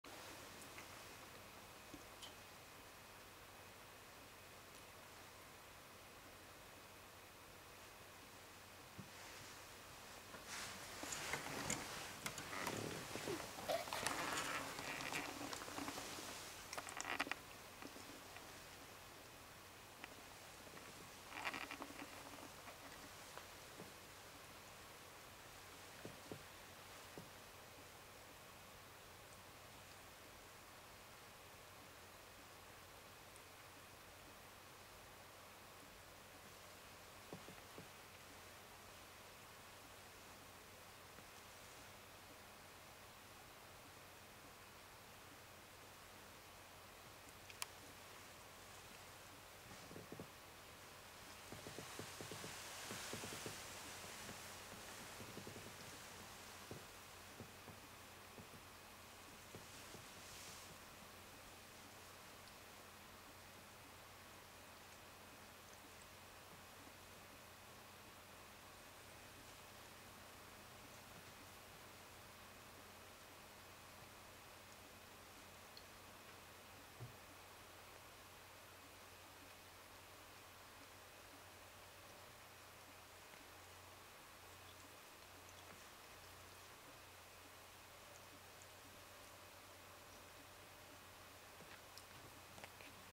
やさしいハナ
一生懸命ミーミーをなめるハナです。ミーミーも安心して眠ってしまいました。